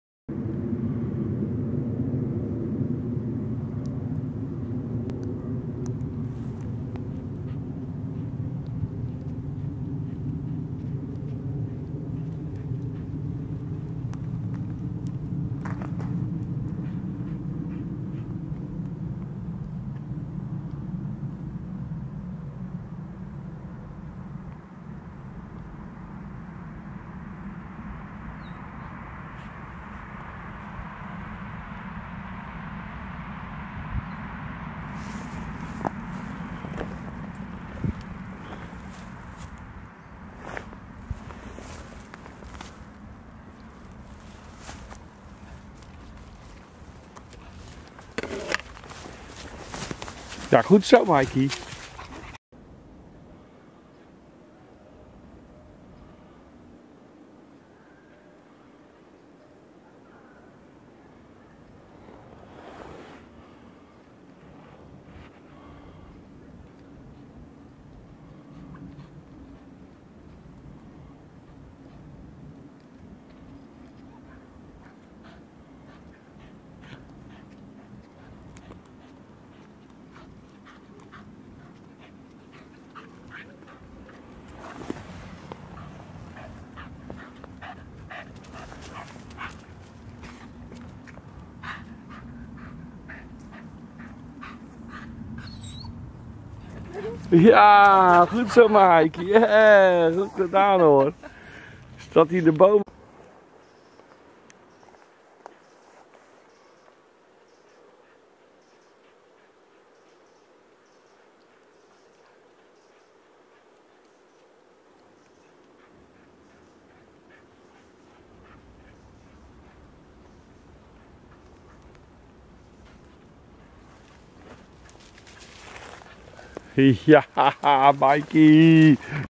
Speuren Maiky 11 02 2015
Vanmiddag de 1e speurles van Maiky. PrachtigMooi om te zien, dat deze 12 jarige rakker ieder spoor steeds beter gaat speuren. Maiky kreeg er steeds meer lol in en werd steeds fanatieker..NatuurSpeurtalentje..HondenTrots op Maiky en Jacolien.